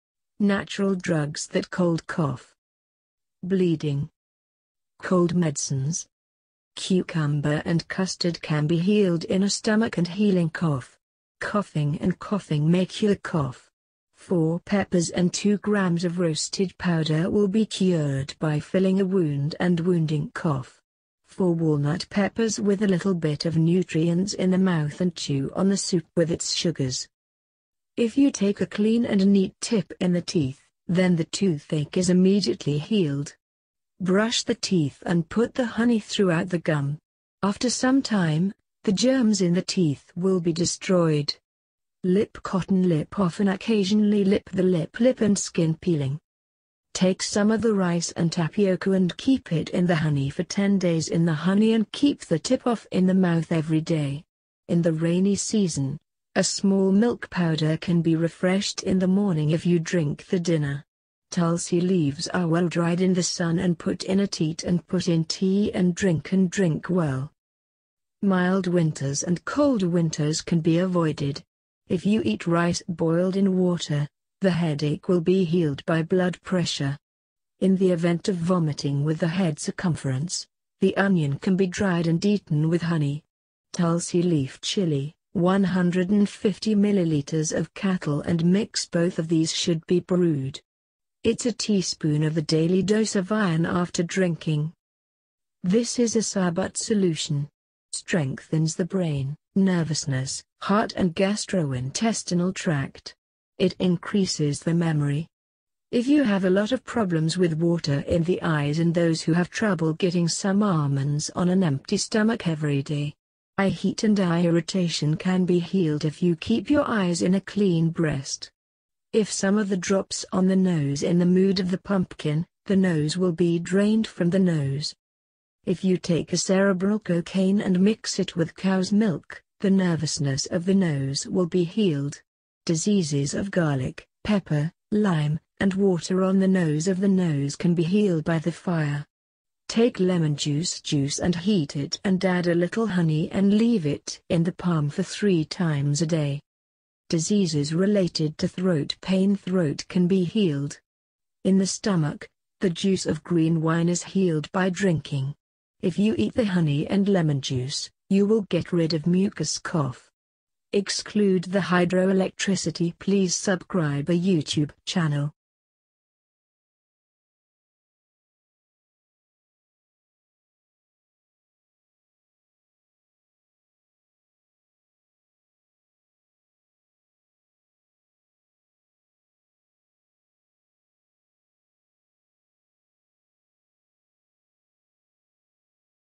Natural Drugs That Cold Cough Bleeding Cold medicines
Natural Drugs That Cold Cough Bleeding
Cold medicines
Cucumber and custard can be healed in a stomach and healing cough. Coughing and coughing may cure the cough. Four peppers and two grams of roasted powder will be cured by filling a wound and wounding cough. Four walnut peppers with a little bit of nutrients in the mouth and chew on the soup with its sugars.
If you take a clean and neat tip in the teeth, then the toothache is immediately healed. Brush the teeth and put the honey throughout the gum. After some time, the germs in the teeth will be destroyed. Lip cotton lip often occasionally lip the lip lip and skin peeling.

Take some of the rice and tapioca and keep it in the honey for ten days in the honey and keep the tip off in the mouth every day. In the rainy season, a small milk powder can be refreshed in the morning if you drink the dinner. Tulsi leaves are well dried in the sun and put in a teat and put in tea and drink and drink well.
Mild winters and cold winters can be avoided. If you eat rice boiled in water, the headache will be healed by blood pressure. In the event of vomiting with the head circumference, the onion can be dried and eaten with honey. Tulsi leaf chilli, 150 ml of cattle and mix both of these should be brewed. It's a teaspoon of the daily dose of iron after drinking.
This is a Sarbat solution. Strengthens the brain, nervousness, heart and gastrointestinal tract. It increases the memory. If you have a lot of problems with water in the eyes and those who have trouble getting some almonds on an empty stomach everyday. Eye heat and eye irritation can be healed if you keep your eyes in a clean breast. If some of the drops on the nose in the mood of the pumpkin, the nose will be drained from the nose.
If you take a cerebral cocaine and mix it with cow's milk, the nervousness of the nose will be healed. Diseases of garlic, pepper, lime, and water on the nose of the nose can be healed by the fire. Take lemon juice juice and heat it and add a little honey and leave it in the palm for 3 times a day.
Diseases related to throat pain throat can be healed. In the stomach, the juice of green wine is healed by drinking. If you eat the honey and lemon juice, you will get rid of mucous cough. Exclude the hydroelectricity.